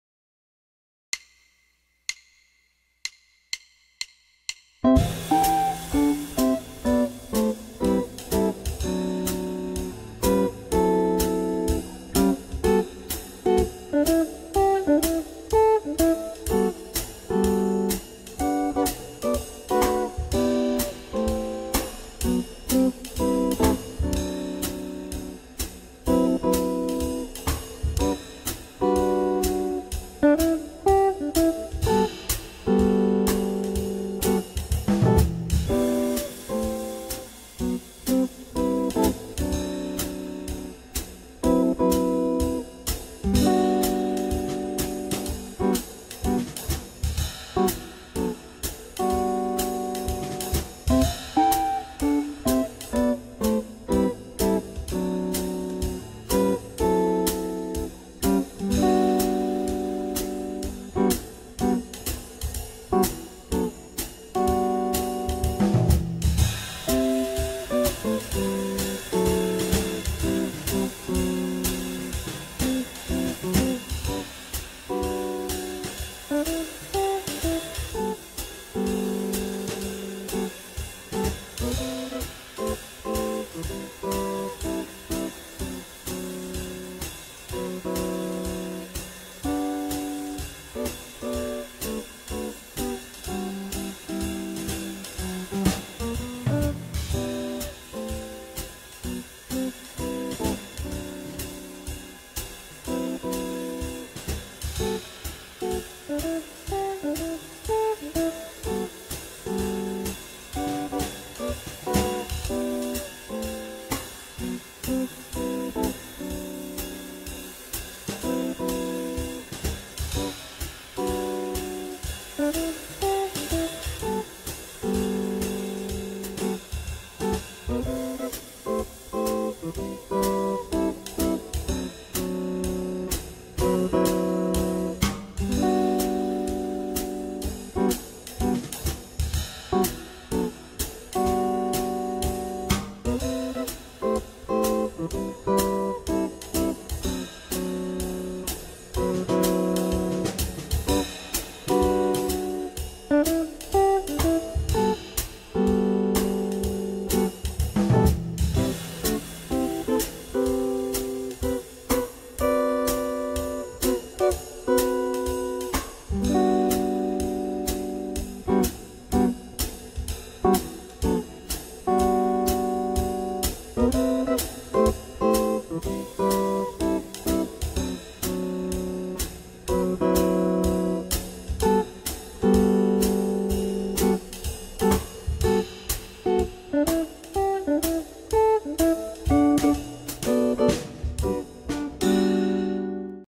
Am7-D7-Gmaj7 (2-2-4 compassos) Jazz (Backing Track Para Baixistas)
Backing Track Para Baixistas - Acordes Am7, D7 e Gmaj7 com 2, 2 e 4  compassos por acorde respectivamente em Jazz sem o baixo.